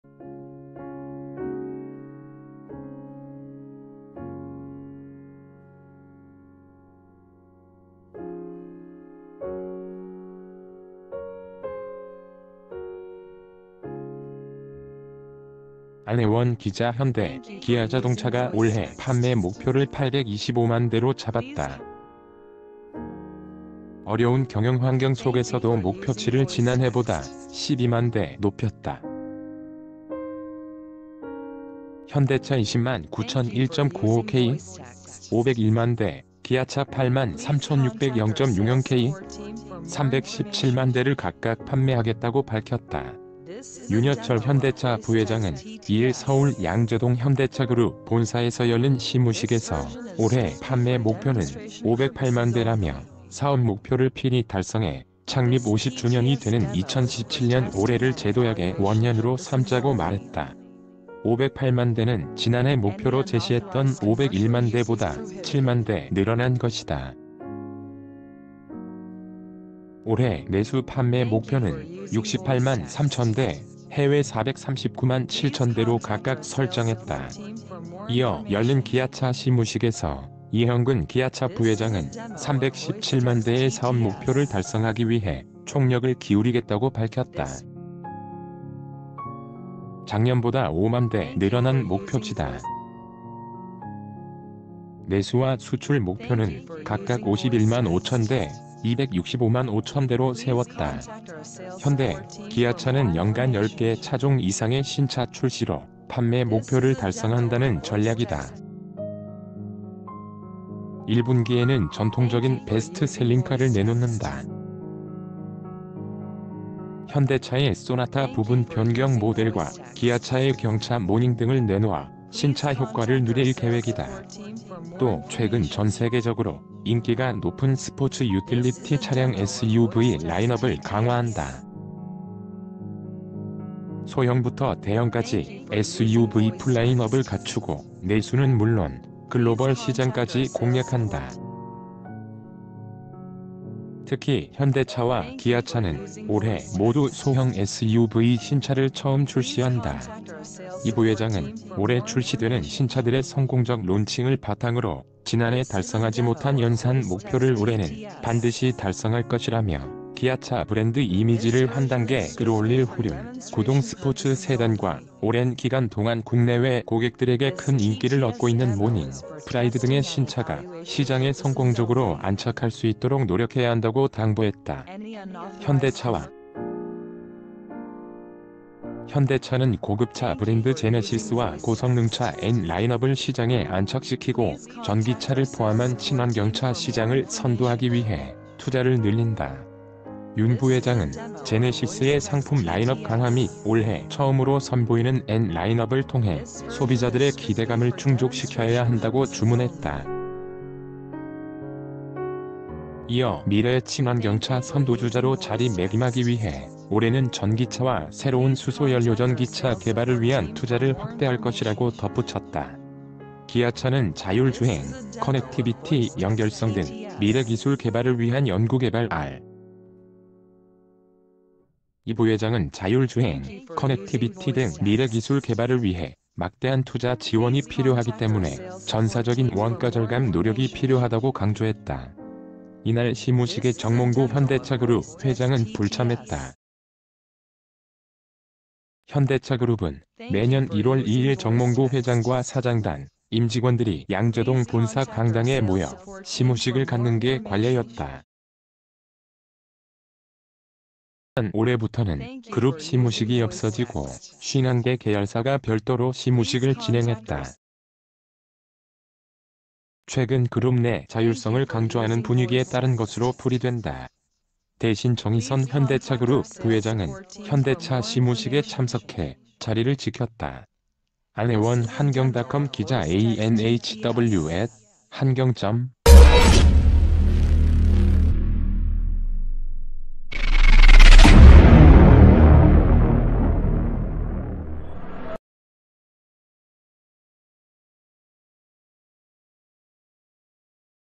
현대·기아차 "올해 825만대 팔겠다"…작년보다 12만대 늘어
현대·기아차 "올해 825만대 팔겠다"…작년보다 12만대 늘어 [ 안혜원 기자 ] 현대·기아자동차가 올해 판매 목표를 825만대로 잡았다. 어려운 경영환경 속에서도 목표치를 지난해보다 12만대 높였다. 현대차(209,000 +1.95%)는 501만대, 기아차(83,600 +0.60%)는 317만대를 각각 판매하겠다고 밝혔다.윤여철 현대차 부회장은 2일